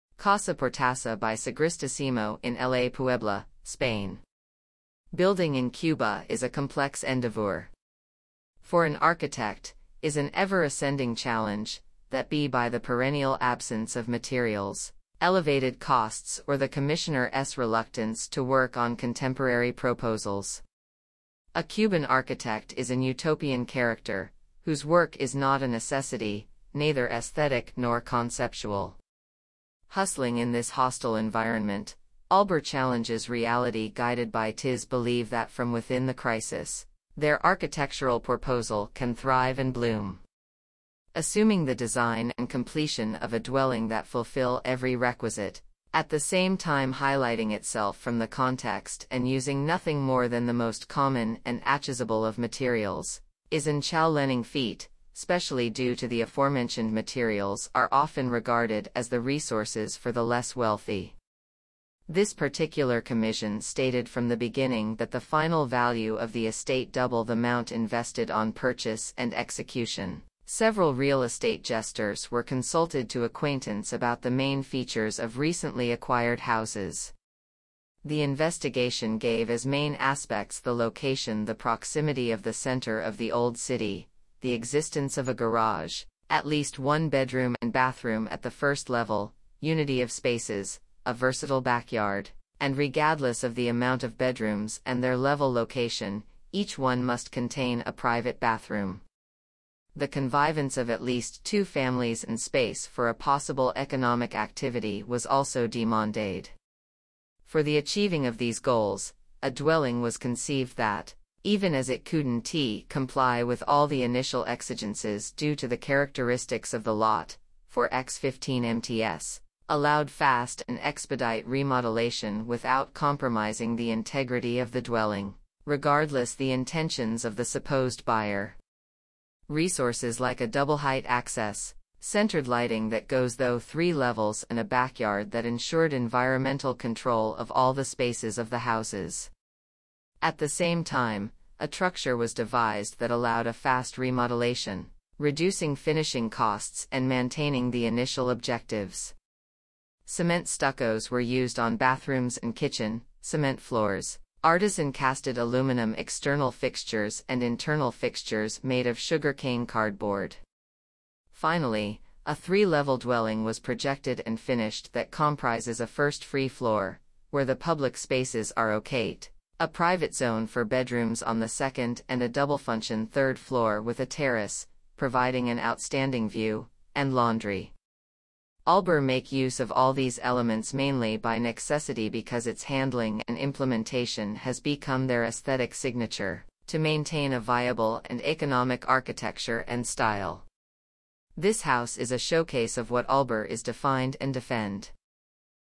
Casa Portassa By Sagristà-Simó In LA PUEBLA, SPAIN
Building in Cuba is a complex endavour. For an architect, is an ever ascending challenge, that be by the per-ennial absence of materials, elevated costs or the commissioner´s reluctance to work on contemporary pro-posals. A cuban architect is an utopian character, whose work is not a necessity, neither aesthetic nor con-ceptual. Hustling in this hostile environment, Albor challenges reality guided by tis believe that from within the crisis, their architectural porposal can thrive and bloom.

Archi Studio

Architects: Sagristà-Simó
Area:
348 m²
Photographs:Carlos Manuel González Baute
Manufacturers:  Fullana, Fusteria Rian, Hormigones Farrutx, Mármoles Valcaneras, Pastor Structures & Architecture
Technical Architect: Joan Palou Cantallops
Construction: Miquel Simó Plomer
Promotor: Margalida Serra Rebassa, Miquel Simó Plomer
City: La Puebla
Country: Spain